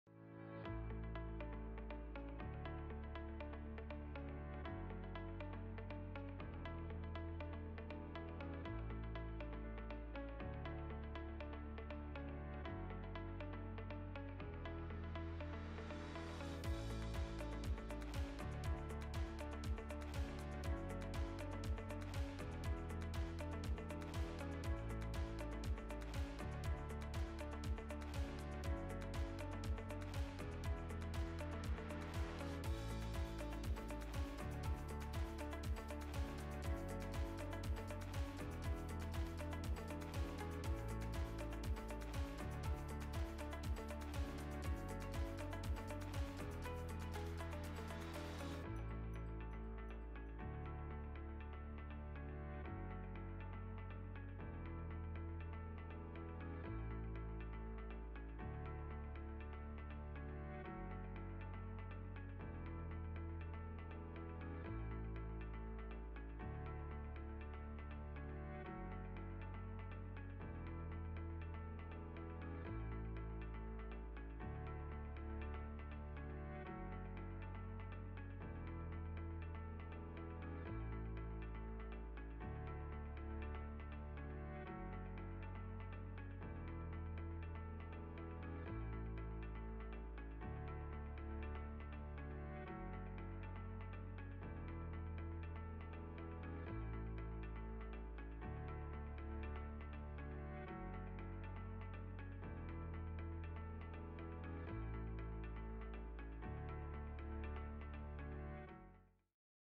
Airframe Tent vs Steel Pole Tent
An inflatable airframe makes pitching your tent quicker and easier. up to 10x easier. Watch the pitch comparison of our Airforce 1 V2 airframe tent and its steel pole equivalent our Titan.